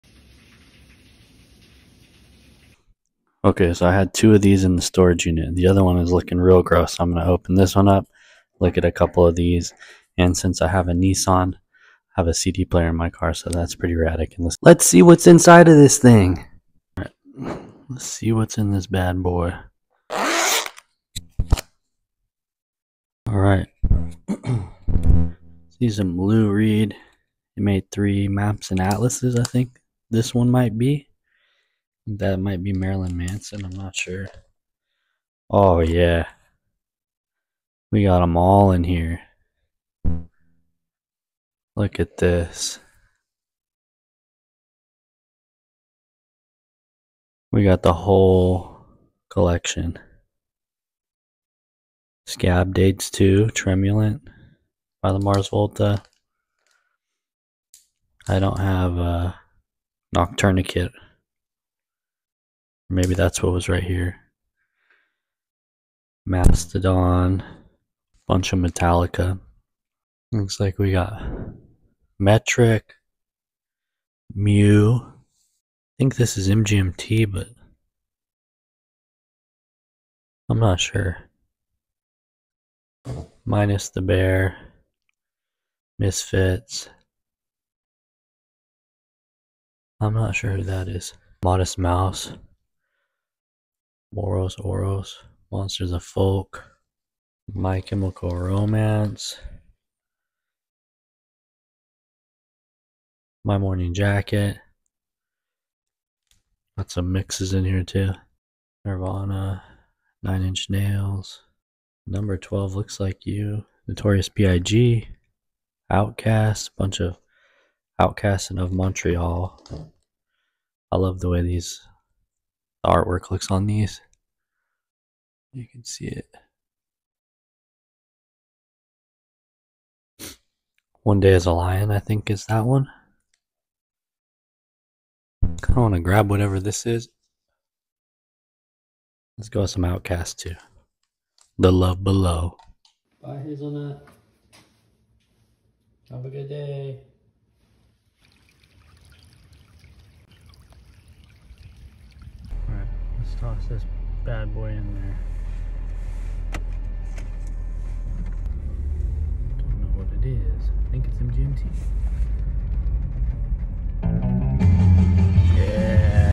Boring vlog 1
Look at some old cds with me. Why is the rainwater soapy?